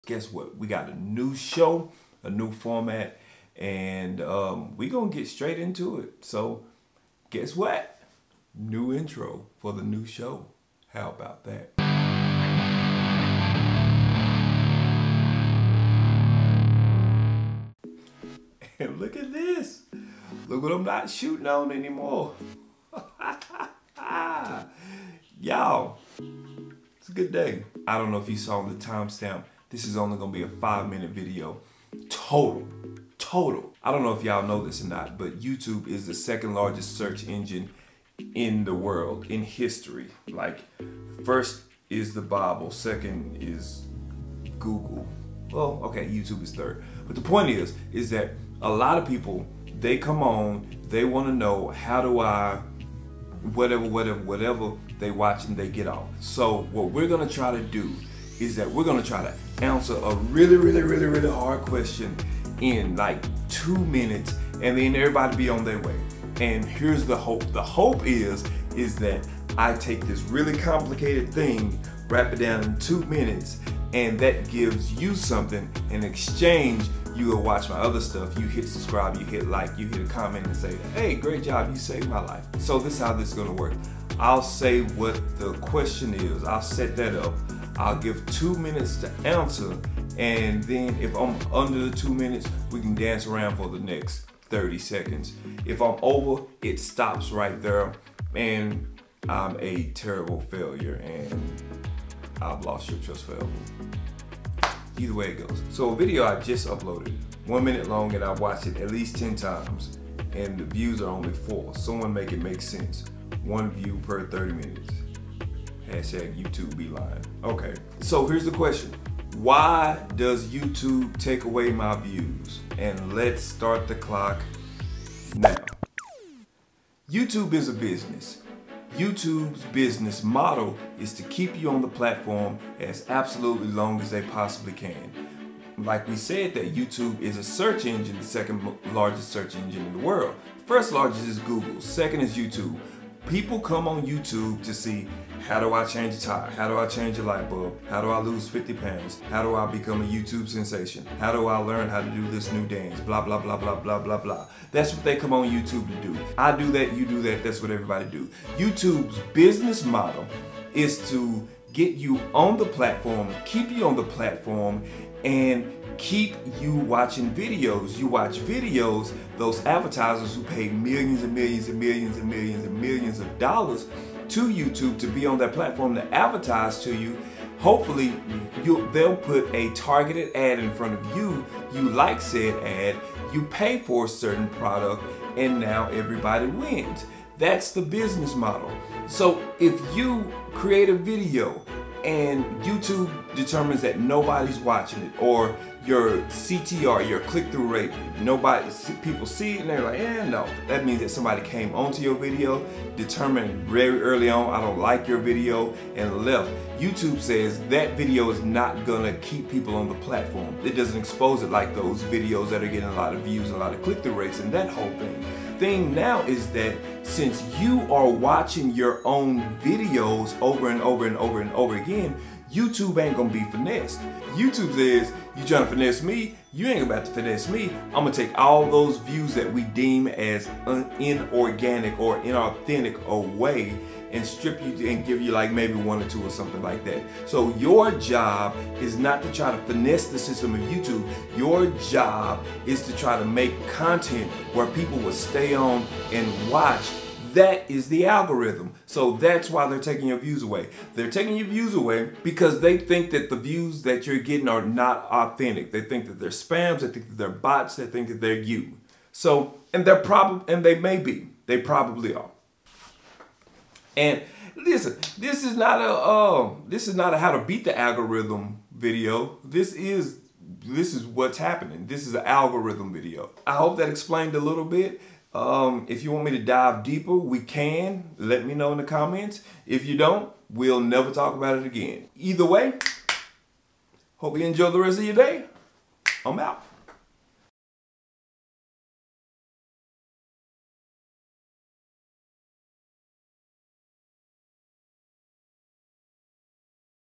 Why Do You Lose Views Sometimes : Small Youtuber Advice for 2020
Hey new Youtubers? Maybe you’re finally taking youtube serious and really cranking out those videos.  You know your family/friends not watching your videos right now, so you decided to give it a little help and watch your videos yourself...to give it a little kickstart 😉. Have you ever looked at your video at least 5-10 times and checked it and noticed you only had about 3 views?

Orrrrrr just DM me. I’ll respond. Probably 🤷🏾‍♂️